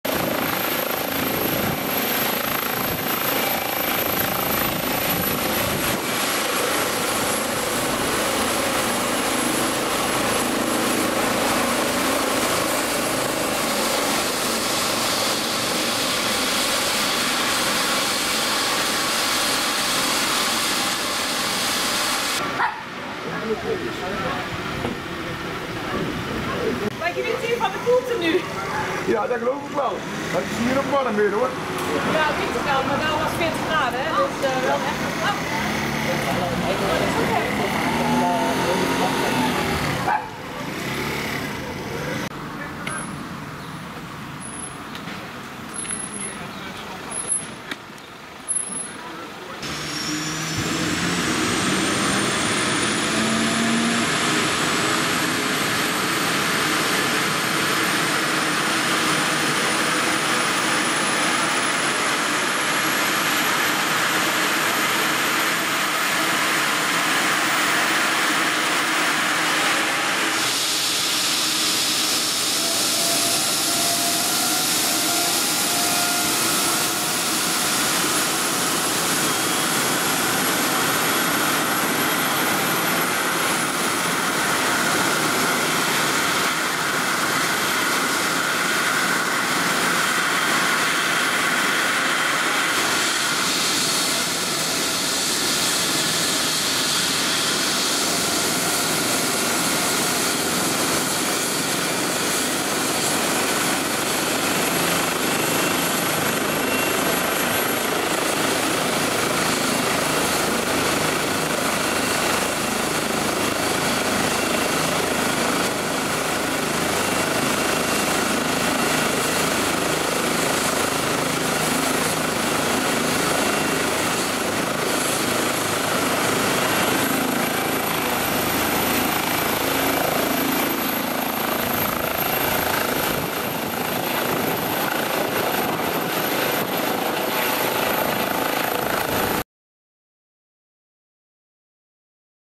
Lifeliner 3 en Politie Oss met spoed naar en van een Medische Noodsituatie aan de Molenstraat in Oss
- In deze video ziet u:
Lifeliner 3 Volkel
Politie Oss